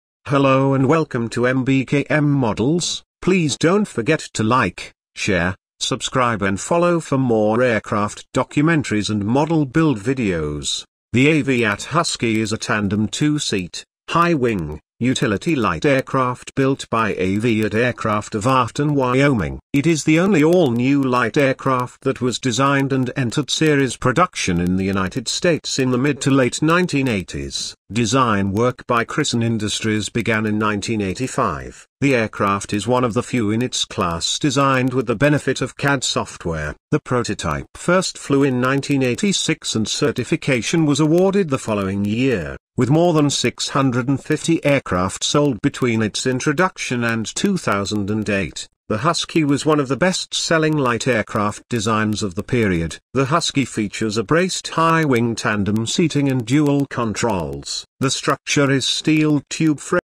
Aviat Husky Seaplane Documentary Short
The Aviat Husky is a tandem two-seat, high-wing, utility light aircraft built by Aviat Aircraft of Afton, Wyoming. It is the only all-new light aircraft that was designed and entered series production in the United States in the mid-to-late 1980s. Design work by Christen Industries began in 1985. The aircraft is one of the few in its class designed with the benefit of CAD software. The prototype first flew in 1986, and certification was awarded the following year. With more than 650 aircraft sold between its introduction and 2008, the Husky was one of the best-selling light aircraft designs of the period

Aviat Husky Seaplane Documentary